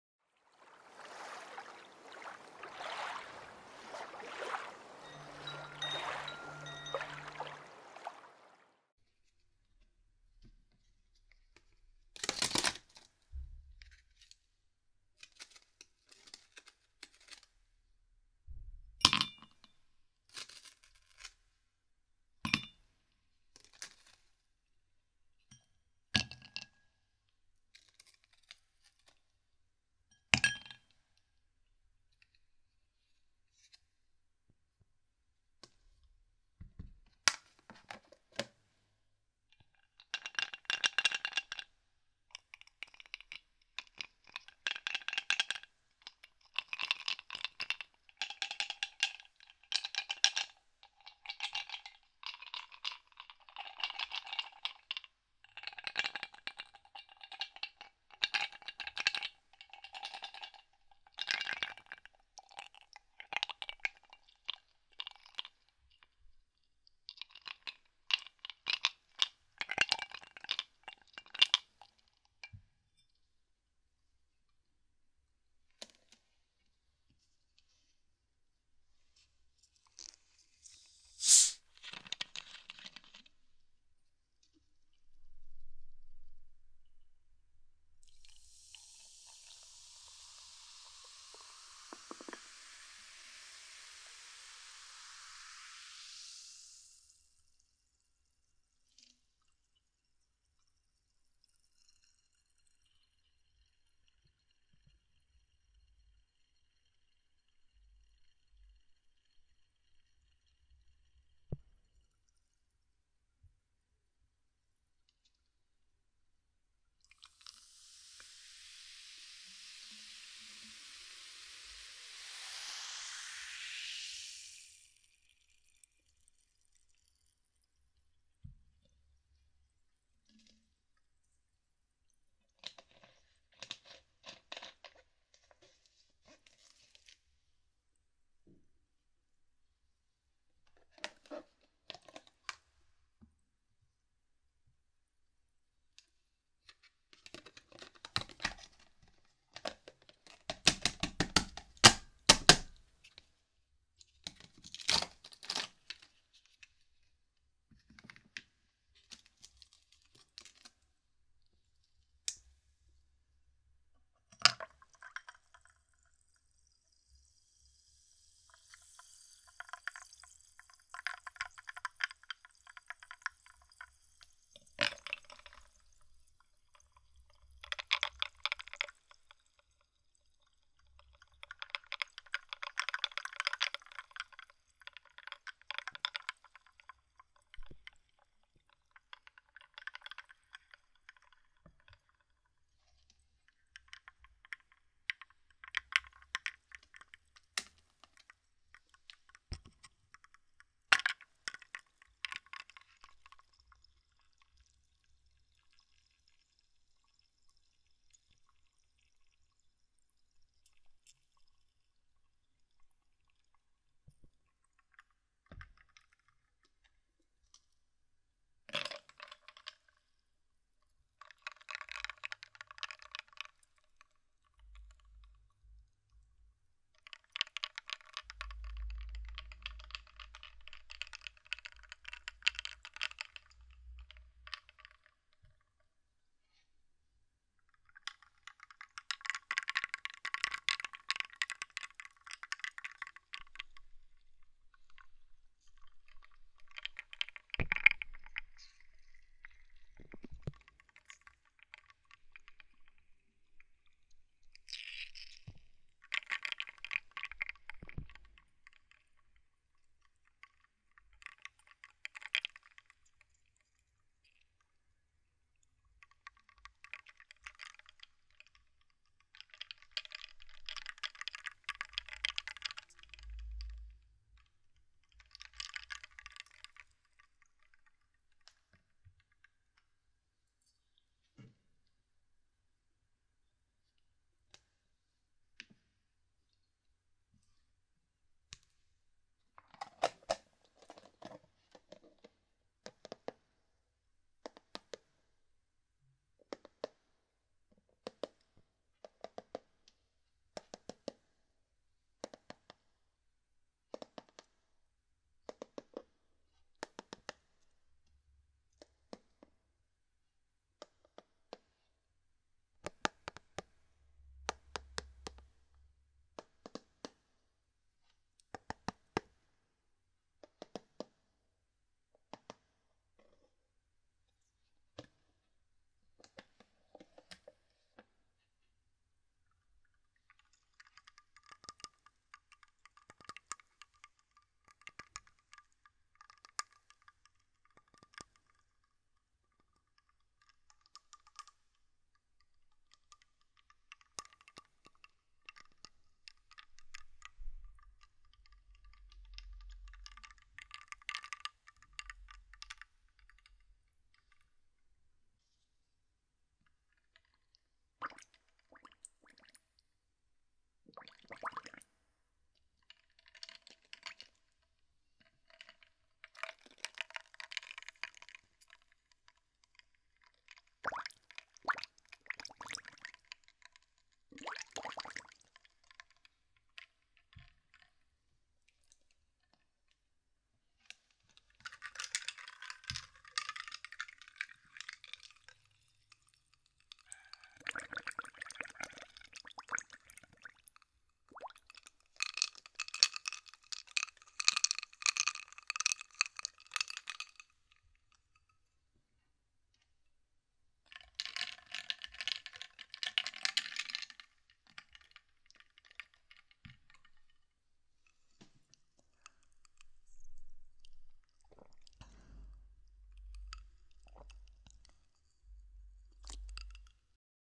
【ASMR】冰块 | 汽水
依旧请戴上你的耳机哦~
建议裹紧小杯子再收看哦~
这期主要是冰块的声音啦，音频版可以在网易云-双马尾馨儿-电台中收听哦~